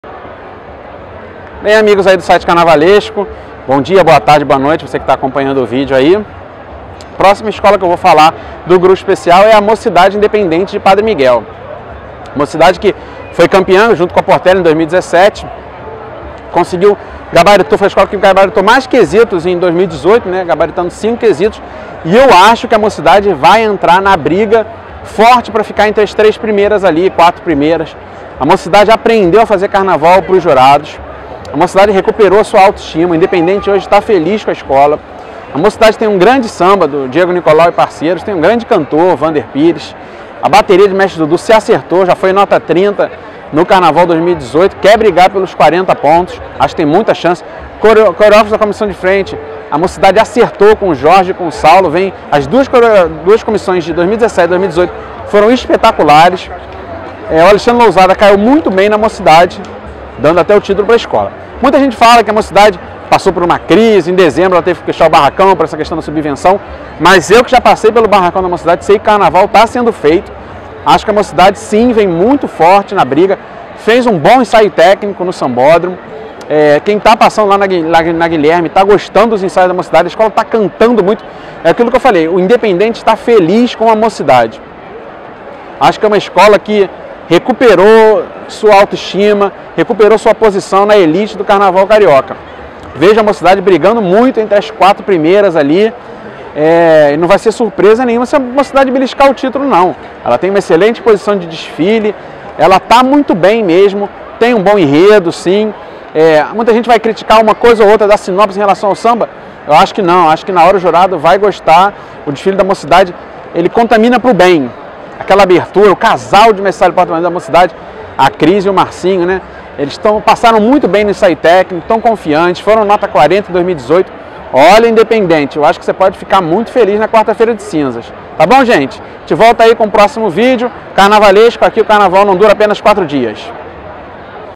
Alberto João: 'Minha expectativa para o desfile da Mocidade no Carnaval 2019'
Alberto João: 'Minha expectativa para o desfile da Mocidade no Carnaval 2019'. Vídeo feito pelo site CARNAVALESCO